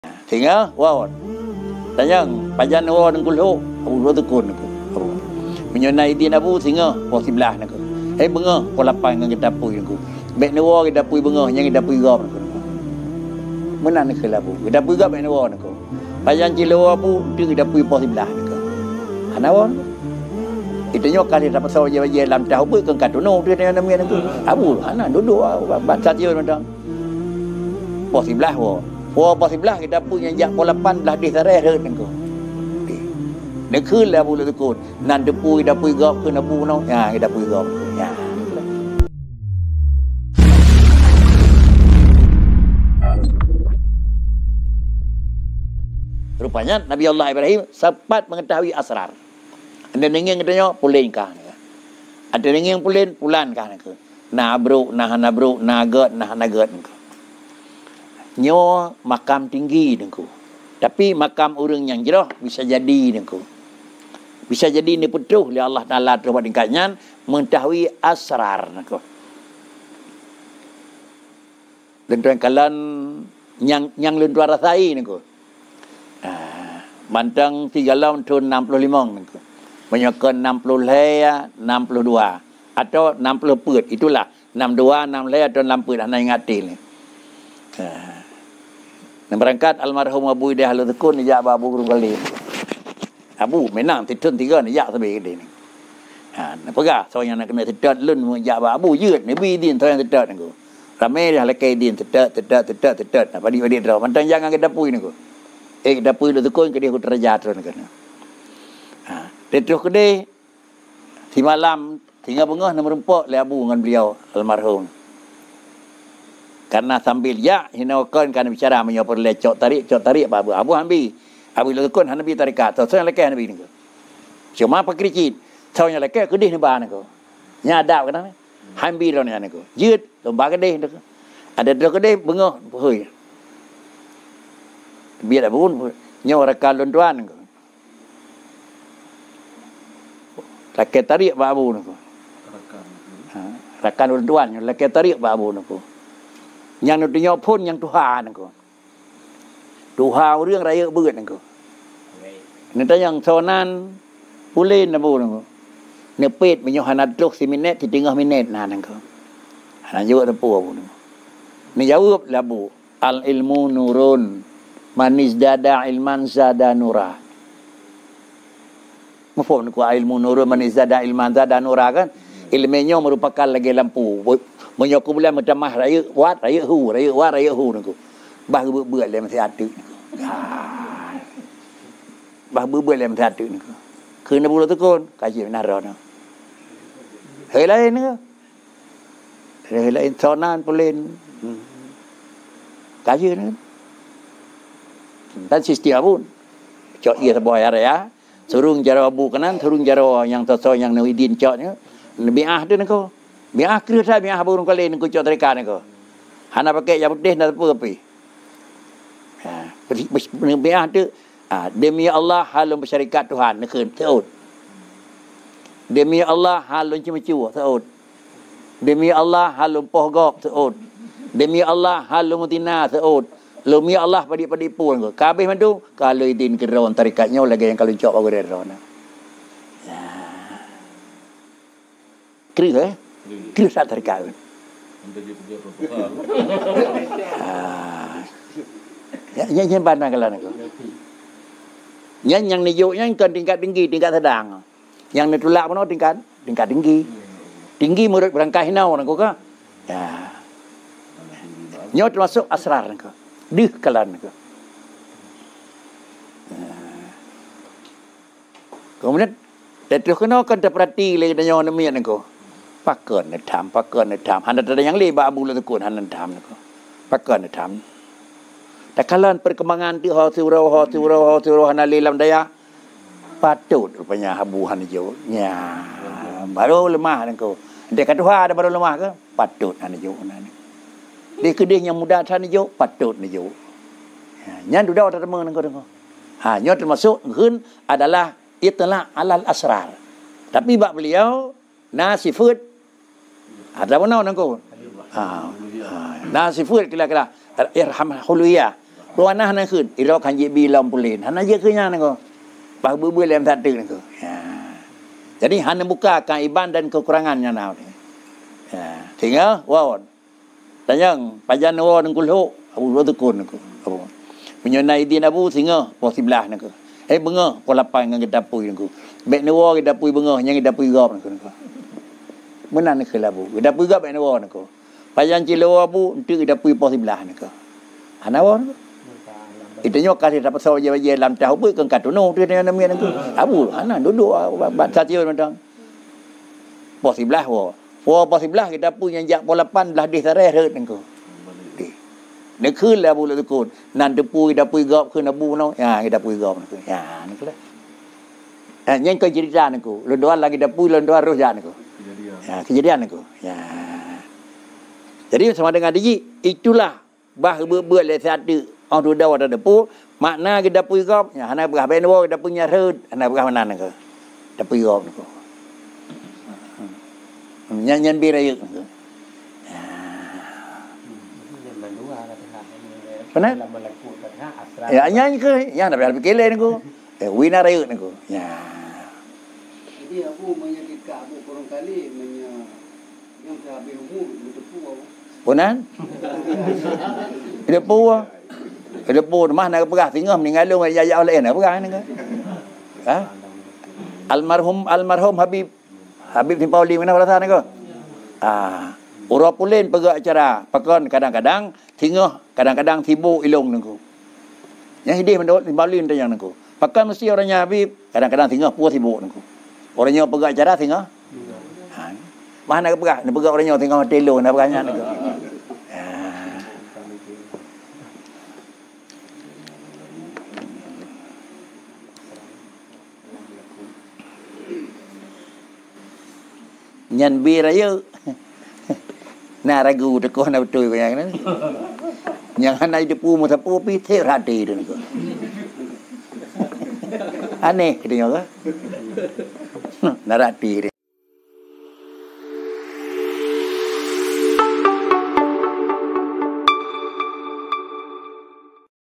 MAQAM ASRAR || ABU MANAN BLANG JRUEN
Pengajian rutin kitab Syarah al-Hikam & Fiqh I'anah At-Talibin
Setiap malam jum'at oleh Al-Mukarram Abu Manan langsung dari kediaman beliau.

Silahkan isi pertayaan anda dikolom komentar, Insya Allah akan dipertanyakan pada pengajian selanjutnya..